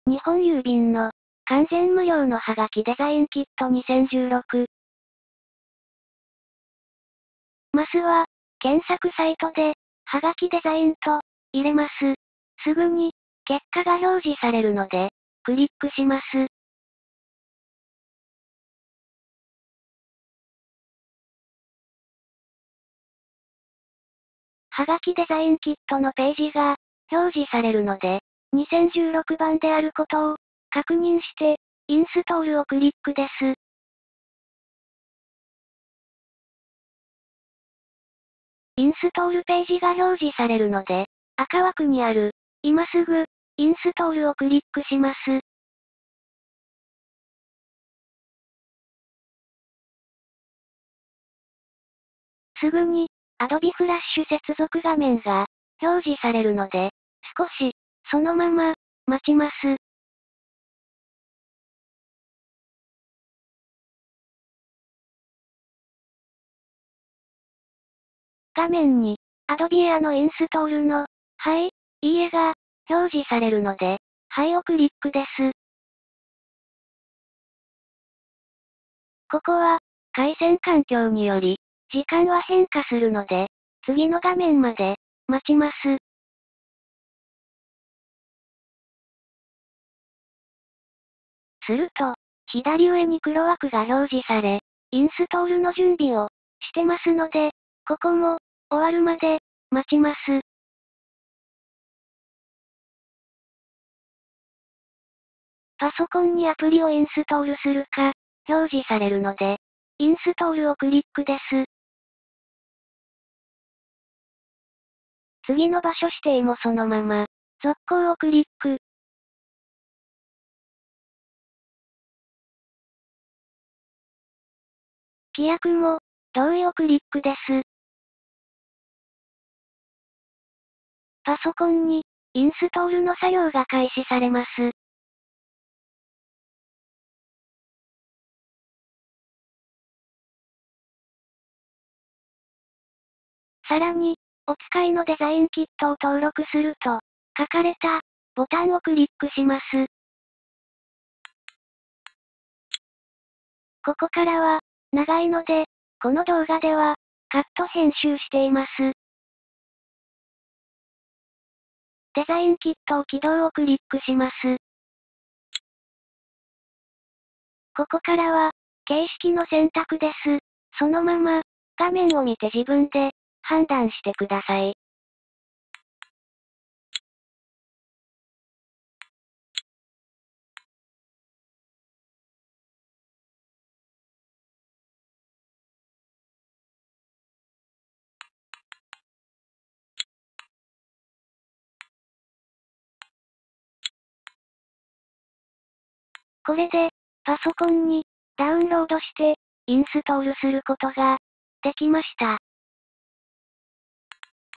はがきデザインきっと2016ダウンロードとインストール
はがきデザインきっと2016のダウンロードとインストール
日本郵便の完全無料の年賀状作成ソフト